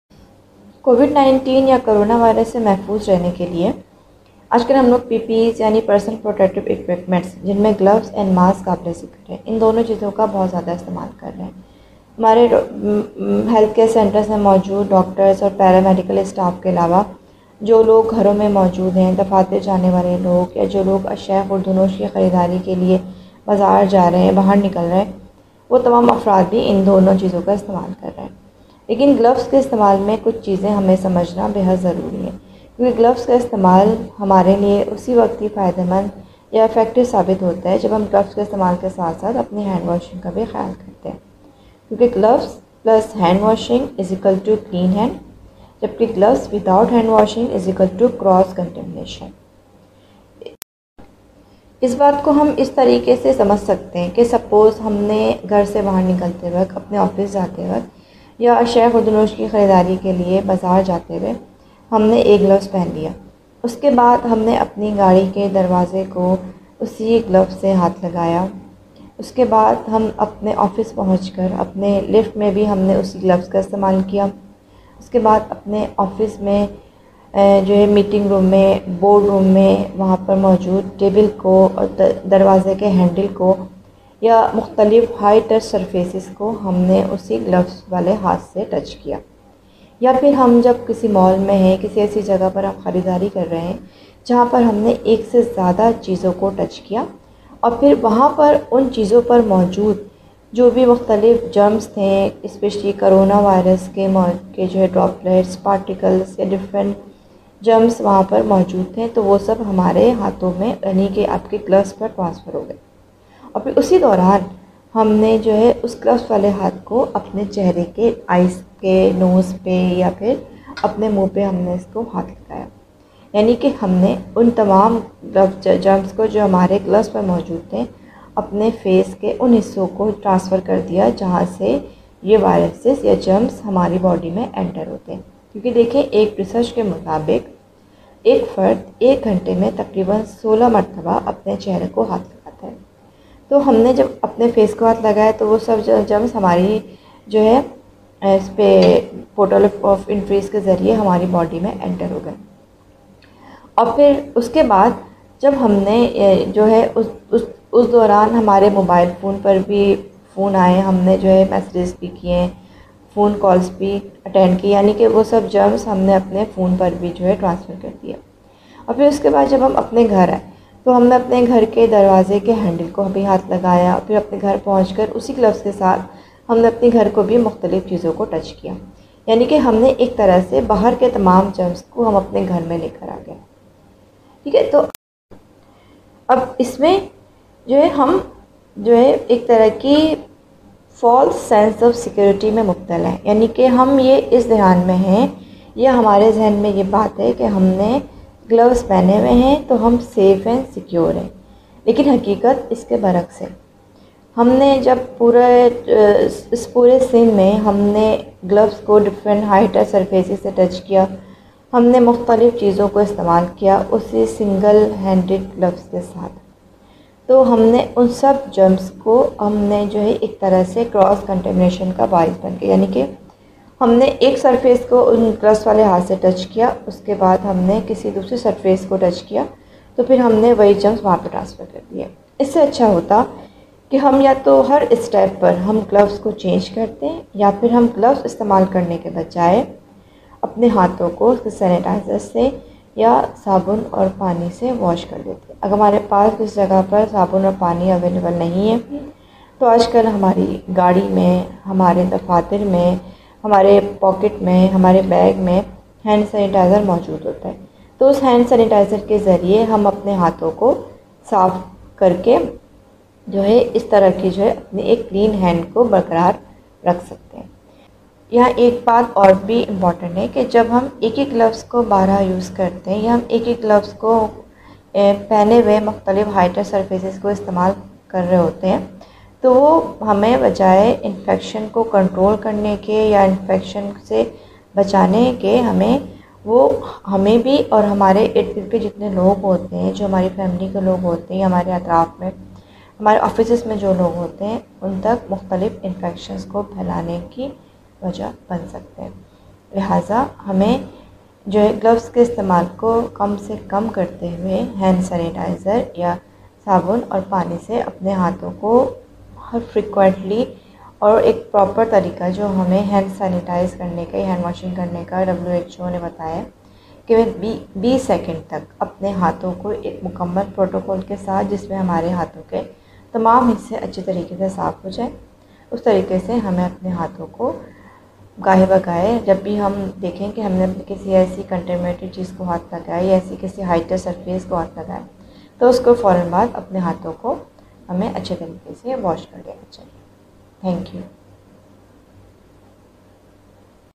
Gloves spread infections
Gloves are not enough; Hand washing is mandatory to avoid cross contamination